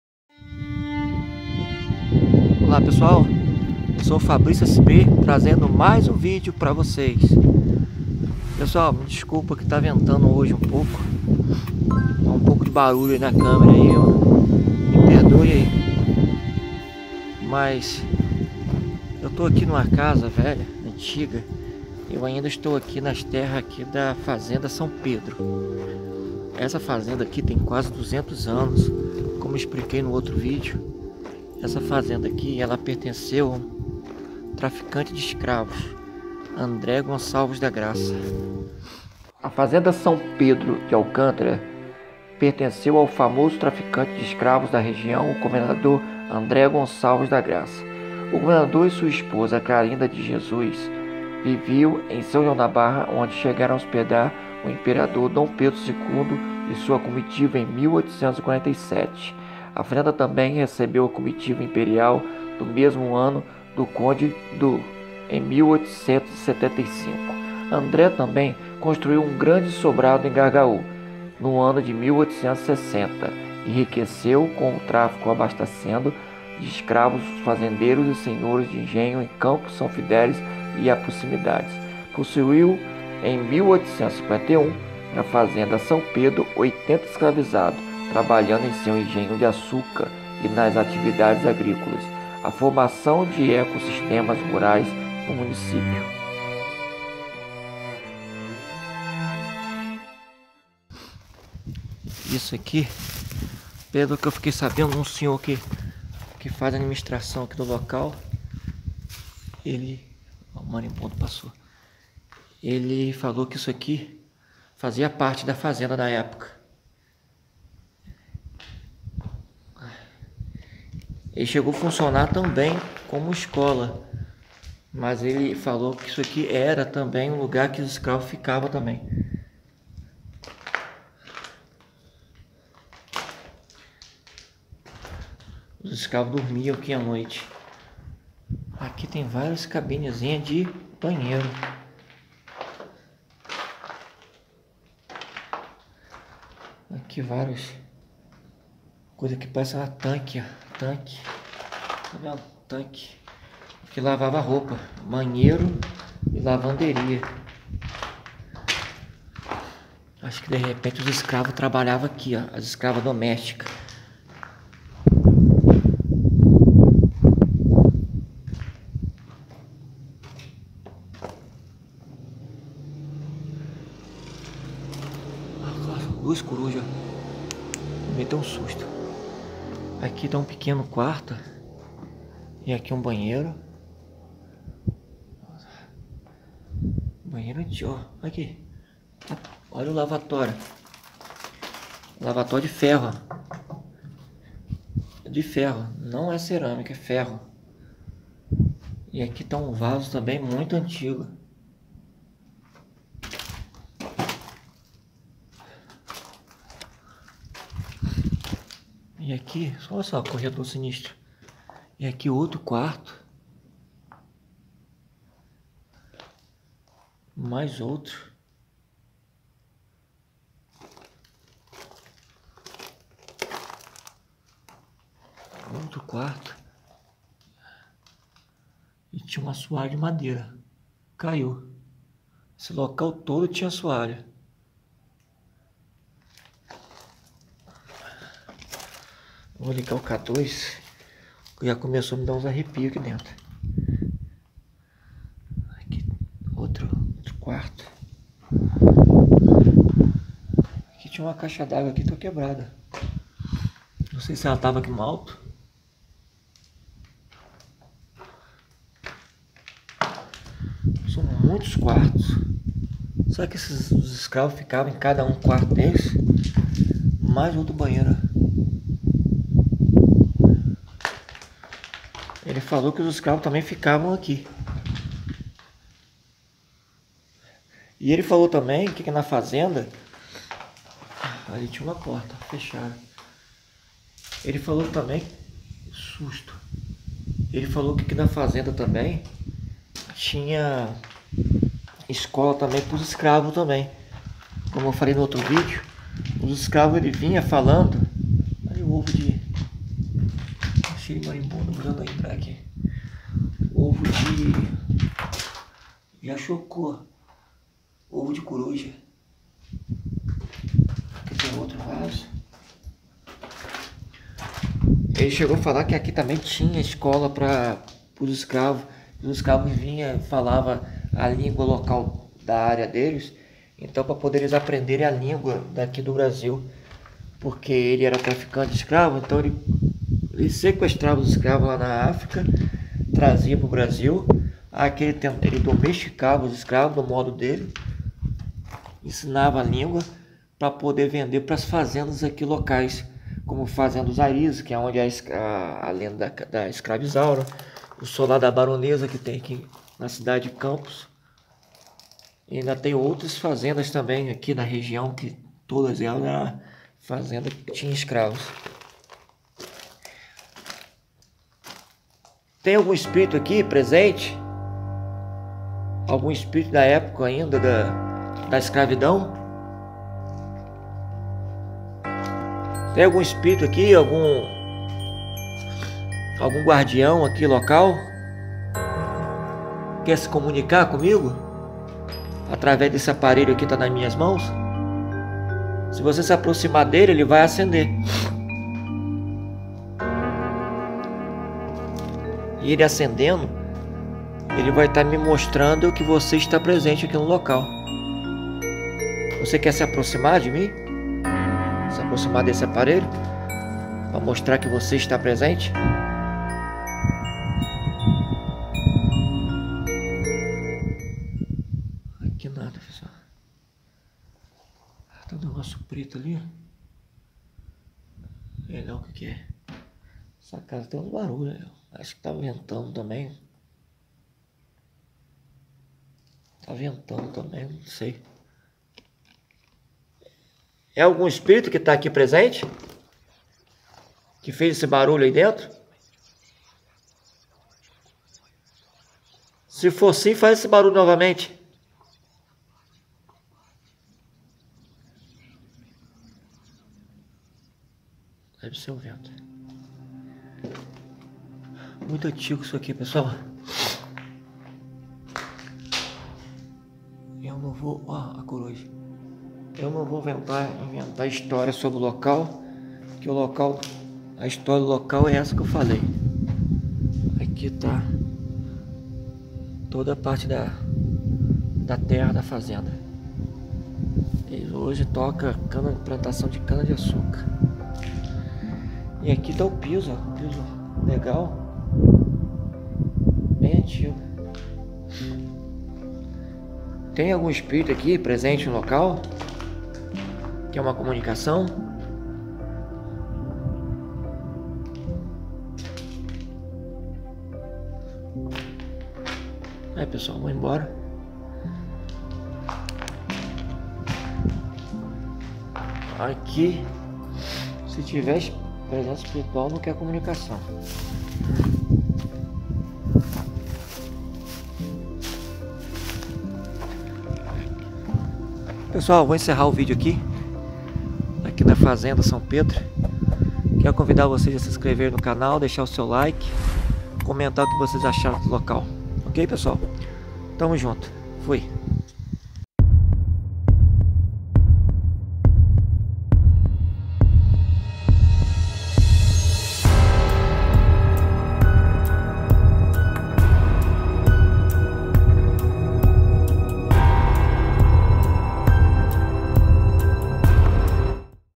CASA CENTENÁRIA 4B4NDON4D4 DA FAZENDA DE UM ESCRAVOCRATA - AQUI ACONTECEU MUITA M4LD4D3
OLÁ PESSOAL. VOLTEI NA FAZENDA SÃO PEDRO QUE PERTENCEU À UM ESCRAVISTA. O LUGAR ERA UMA SENZALA, HOJE BASTANTE MODIFICADA. CHEGOU À FUNCIONAR UMA ESCOLA RURAL.

ACOMPANHA MAIS ESSA AVENTURA.